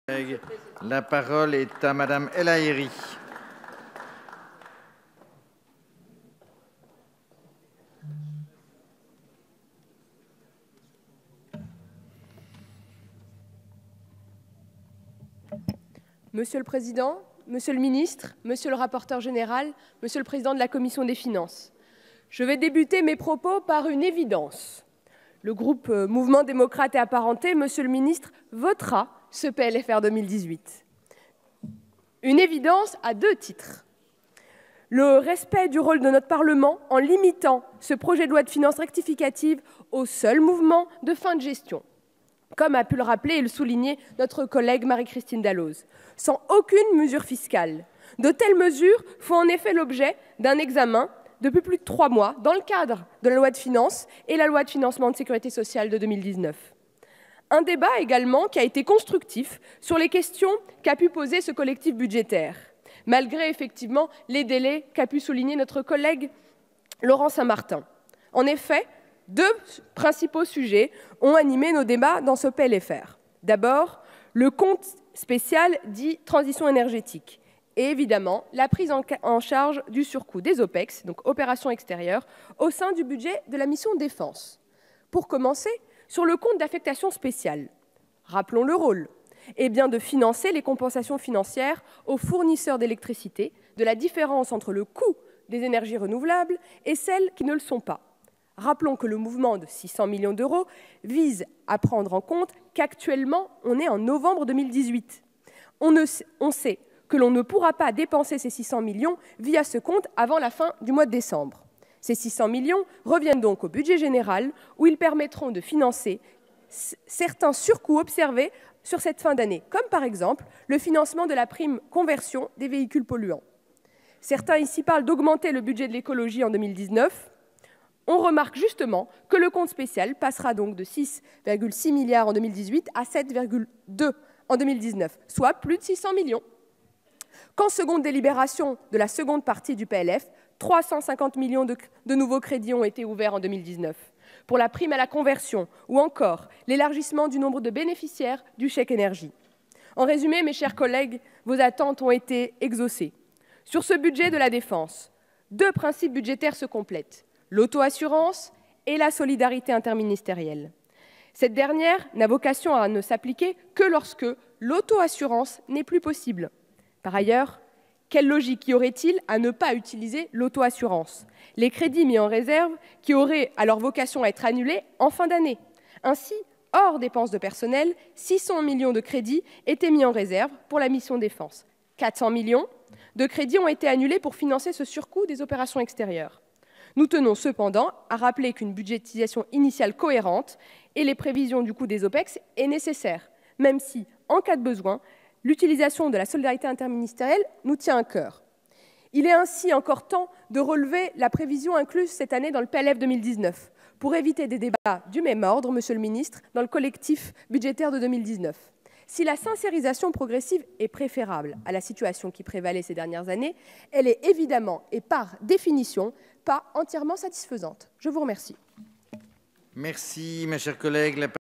Séance publique 28/11/2018: DG PLFR 2018 - lecture définitive
discussion générale sur le Projet de Loi de Finances Rectificative 2018 en lecture définitive.